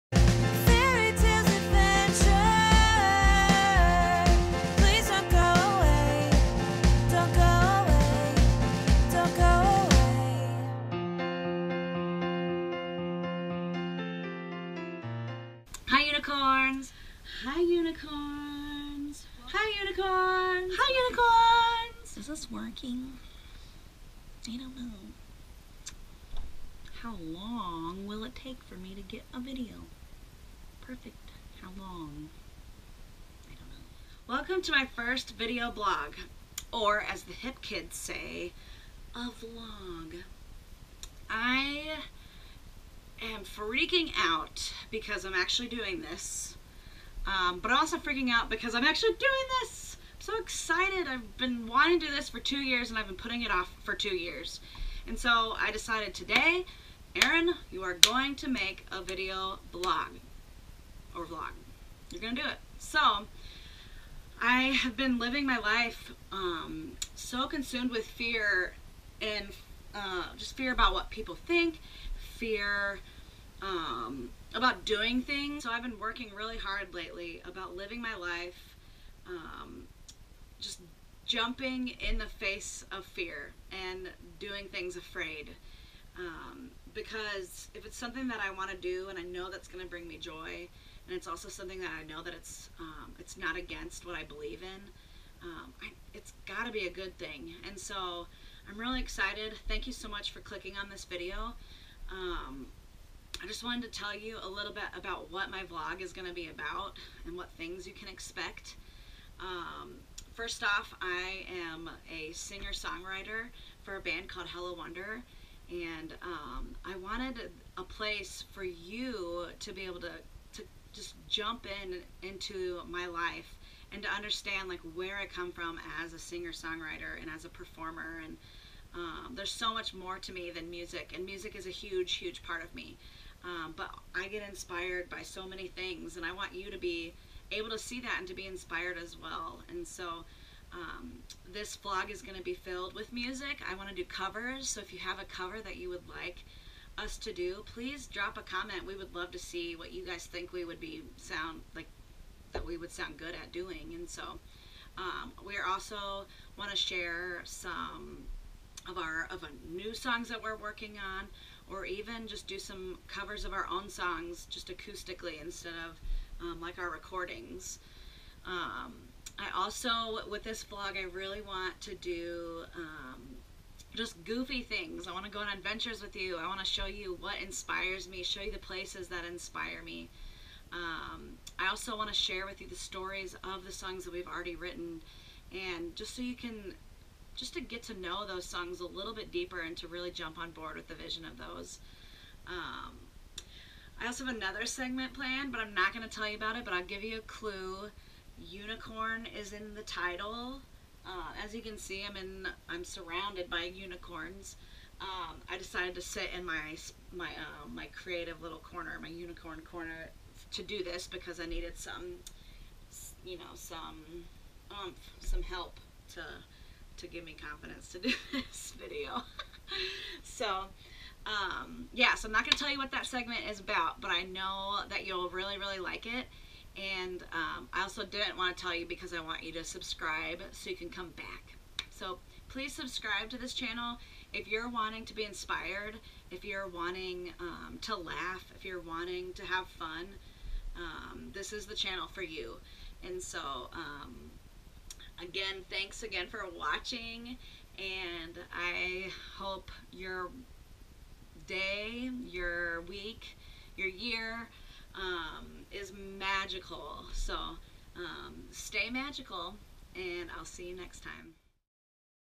Hello Wonder VLOG #1
Join me for my FIRST EVER VLOG! I'm so excited to finally do this and for you all to get to know me on a different level.

STAY MAGICAL 🦄